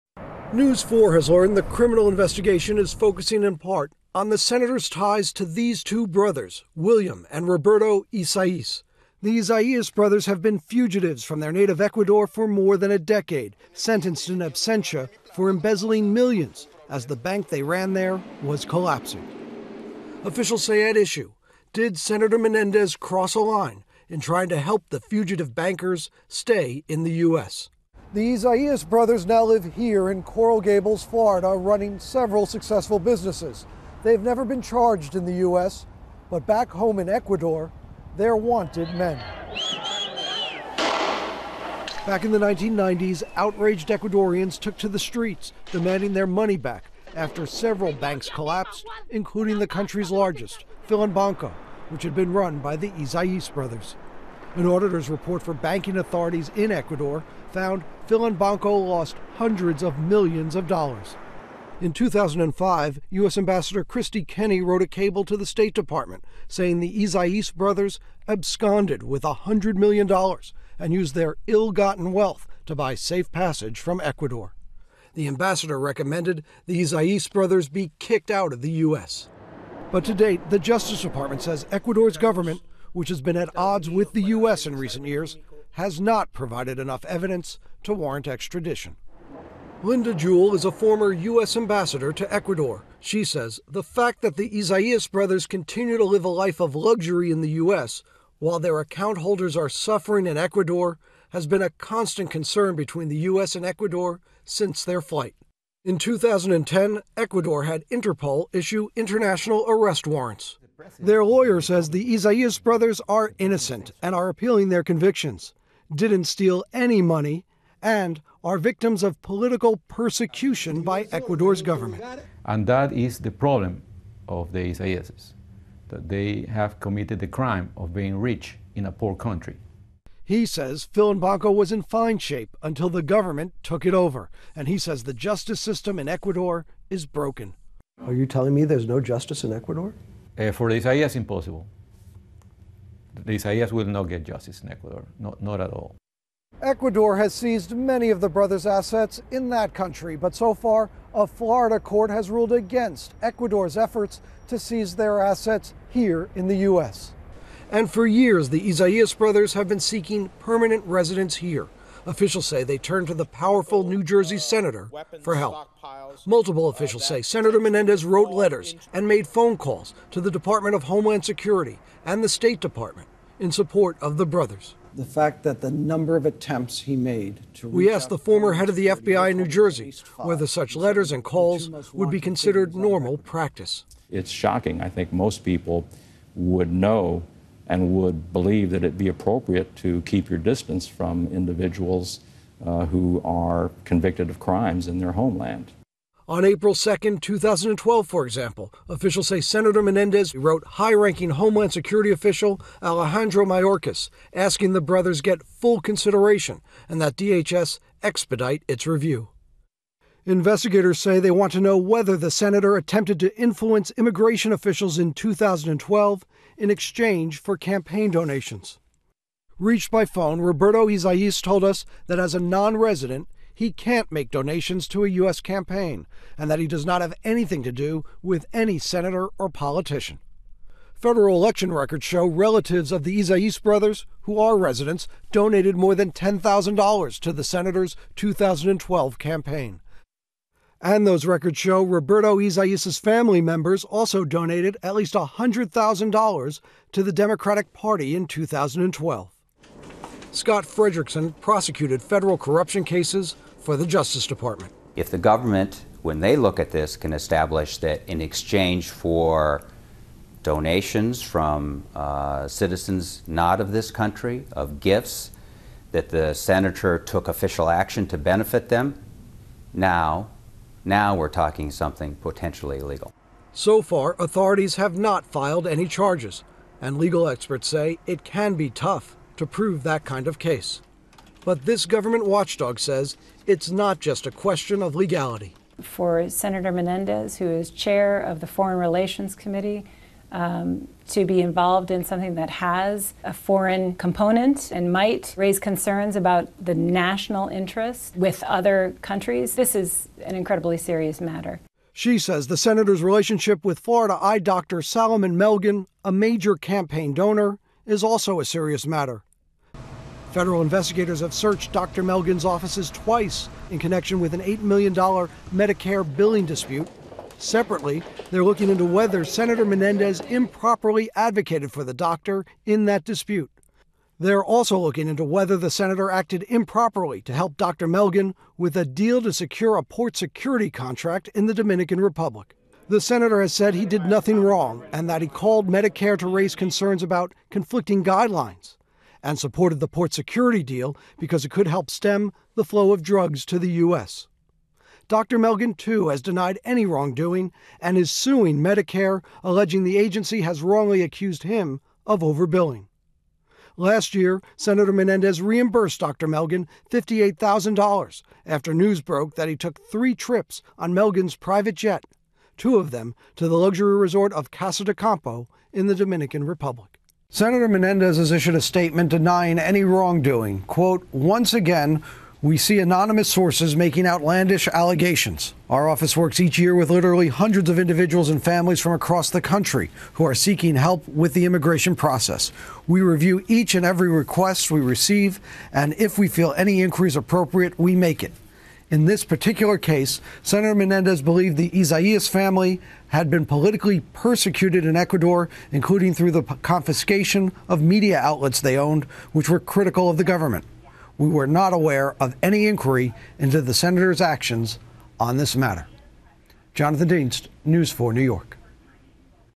FBI Investigating Menendez For Efforts On Behalf Of Fugitive Ecuadorian Bankers
The DOJ is investigating Bob Menendez for aiding two fugitive bankers from Ecuador, according to NBC 4 New York. This is the just latest investigation into the New Jersey Democrat. The Feds are also investigating his relationship to a big campaign donor from Florida.

    The criminal investigation is focusing in part on the senator's ties to William and Roberto Isaias, and whether the senator crossed a line in trying to help the two brothers stay in the United States. The Isaias brothers have been fugitives from their native Ecuador for more than a decade -- sentenced in absentia for embezzling millions as the bank they ran there was collapsing.

    The Isaias brothers now live in Coral Gables, Fla., running several successful businesses, and they have never been charged in the U.S. But back home in Ecuador, they're wanted men.

    Back in the 1990s, outraged Ecuadorians took to the streets demanding their money back after several banks collapsed, including the country's largest, Filanbanco, which had been run by the Isaias brothers. An auditor's report for banking authorities in Ecuador found Filanbanco lost hundreds of millions of dollars.

    In 2005, U.S. Ambassador to Ecuador Kristie Kenney wrote a cable to the State Department saying the Isaias brothers absconded "with $100 million" and "used their ill-gotten wealth to buy safe passage from Ecuador."

    The ambassador recommended the Isaias brothers be kicked out of the United States. But to date, the Justice Department says Ecuador's government, which has been at odds with the U.S. in recent years, has not provided enough evidence to warrant extradition.

    "The fact that the Isaias brothers continue to live a life of luxury in the U.S. while their account holders are suffering in Ecuador has been a constant concern between the U.S. and Ecuador since their flight," said Linda Jewell, a former U.S. Ambassador to Ecuador.

    In 2010, Ecuador had Interpol issue international arrest warrants.

    For years the Isaias brothers have been seeking permanent residence in the United States. Officials say they turned to the powerful New Jersey senator for help.

    Multiple officials say Menendez wrote letters and made phone calls to the Department of Homeland Security and the State Department in support of the brothers. The former head of the FBI in New Jersey says such letters and calls are not the normal practice.

    "It's shocking," said Weysan Dun, the former special agent in charge of the FBI's New Jersey office. "I think most people would know and would believe that it be appropriate to keep your distance from individuals who are convicted of crimes in their homeland."

    On April 2, 2012 for example, officials say, Menendez personally wrote high-ranking Homeland Security official Alejandro Mayorkas, asking that the brothers get "full consideration" and that the Department of Homeland Security "expedite its review."

    Investigators say they want to know whether the senator attempted to influence immigration officials in 2012 in exchange for campaign donations.

    A spokeswoman for Menendez said in a statement: "Our office works each year with literally hundreds of individuals and families from across the country who are seeking help with the immigration process. We review each and every request we receive, and if we feel any inquiry is appropriate, we make it.

    "In this particular case, Sen. Menendez believed the Isaias family had been politically persecuted in Ecuador, including through the confiscation of media outlets they owned which were critical of the government," said the spokeswoman, Tricia Enright. "We are not aware of any inquiry into the senator's actions on this matter."

    Reached by phone, Roberto Isaias said that, as a non-resident of the United States, he cannot make donations to a U.S. political campaign and that he does not have anything to do with any senator or politician.

    Federal election records show relatives of the Isaias brothers who are residents donated more than $10,000 to the senator's 2012 campaign. And records show that the family of Roberto Isaias also donated at least $100,000 to the Democratic party in 2012.

    Campaign donations could be one area of interest to investigators from the Justice Department, said Scott Fredericksen, an attorney who prosecuted federal corruption cases for the Justice Department.

    "If the government, when they look at this, can establish that in exchange for donations from citizens not from this country, of gifts, that the senator took official action that benefited them, now, now we're talking about something potentially illegal," Fredericksen said.